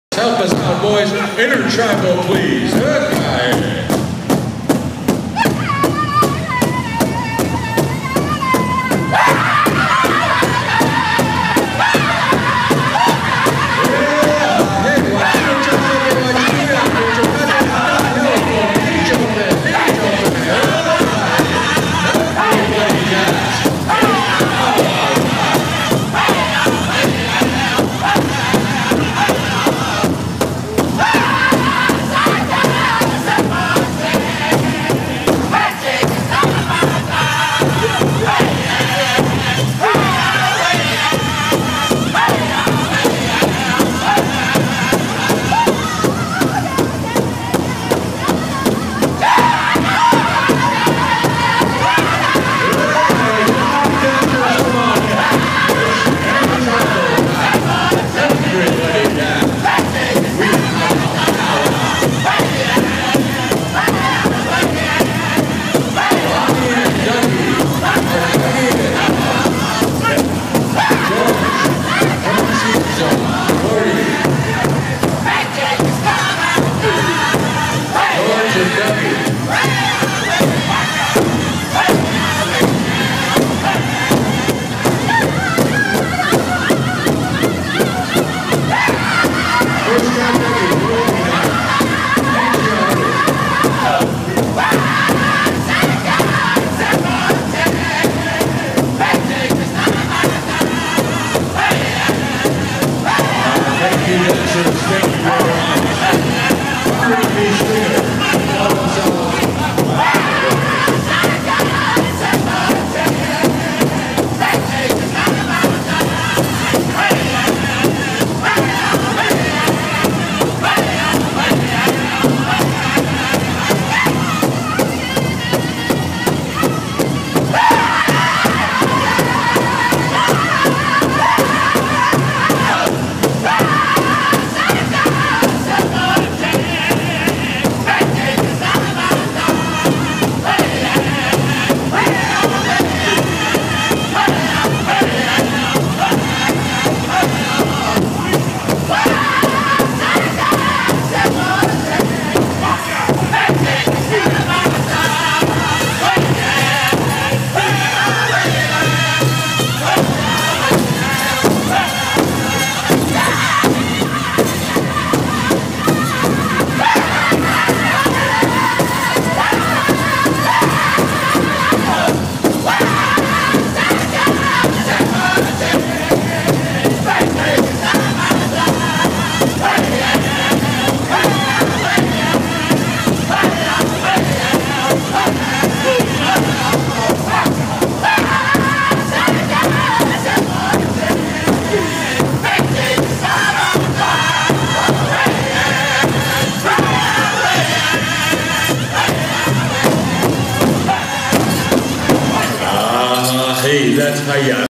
Wild Horse @ Painted HandCasino Powwow 2016
Wild horse jamming an intertribal Saturday afternoon